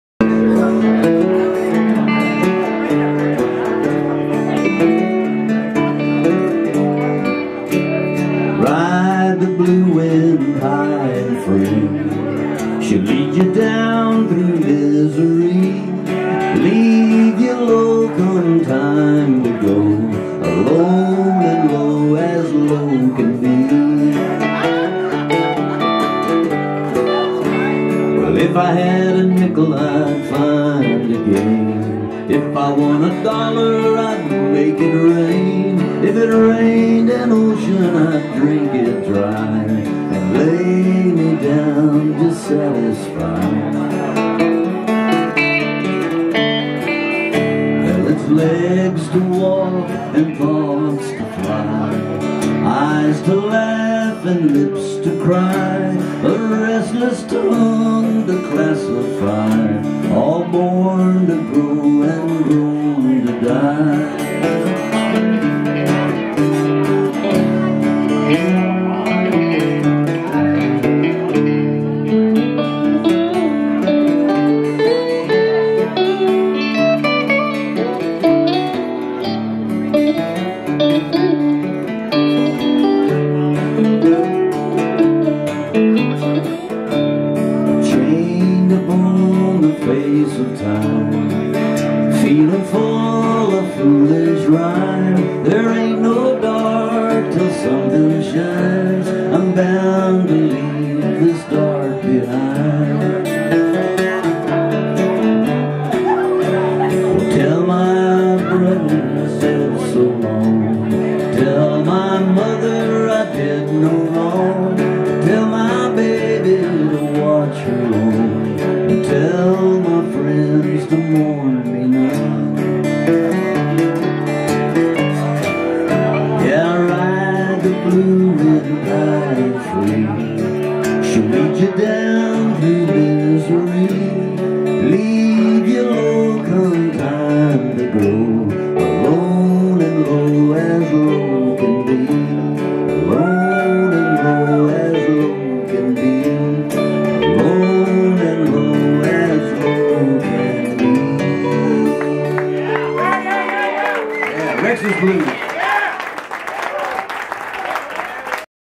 Butch Hancock with Rory
The Towns Van Zandt Tribute at the Grotto Fort Worth